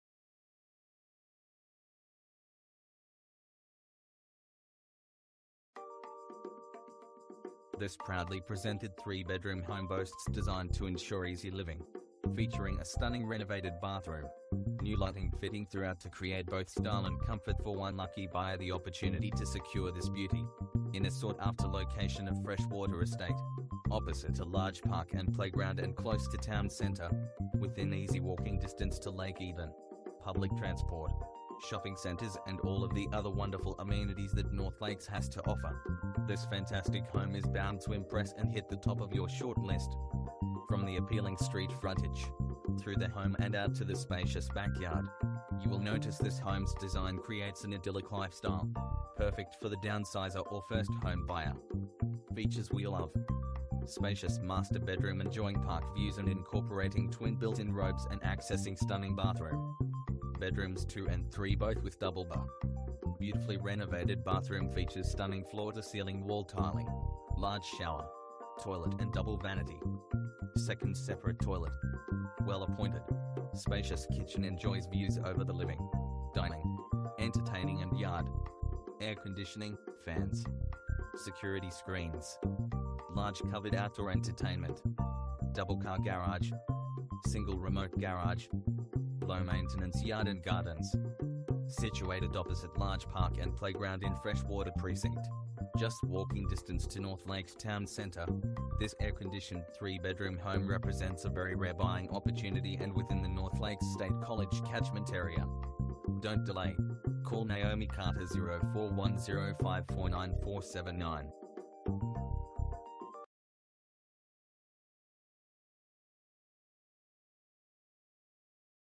7 Raymont Street NORTH LAKES, Queensland
Cute as a button in Sought after Location - This proudly presented 3 bedroom home boasts design to ensure easy living, featuring a stunning renovated bathroom, new lighting fitting throughout to create both style and comfort for one lucky buyer the opportunity to secure this beauty. In a sought after Location of Freshwater estate, opposite a large Park & Playground & close to Town Centre!  Within easy walking distance to Lake Eden, public transport, shopping centres and all of the other wonderful amenities that North Lakes has to offer, this fantastic home is bound to impress and hit the top of your short-list!
From the appealing street frontage, through the home & out to the spacious back yard, you will notice this homes design creates an idyllic lifestyle, perfect for the downsizer or first home buyer.

Features we love:-
- Spacious Master bedroom enjoying park views and incorporating twin built-in robes and accessing stunning bathroom
- Bedrooms 2 and 3 - both with double BIR
- Beautifully renovated bathroom features stunning floor to ceiling wall tiling, large shower, toilet & double vanity
- 2nd  Separate Toilet
- Well appointed, spacious kitchen enjoys views over the living, dining, entertaining and yard
- Air conditioning/Fans
- Security Screens
- Large covered outdoor entertainment
- Double car garage
- Single Remote garage
- Low maintenance yard & gardens

Situated opposite large park and playground in Freshwater precinct, just walking distance to North Lakes Town Centre, this air-conditioned 3 Bedroom home represents a very rare buying opportunity & within the North Lakes State College catchment area.
Don't delay, call Naomi Carter - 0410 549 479.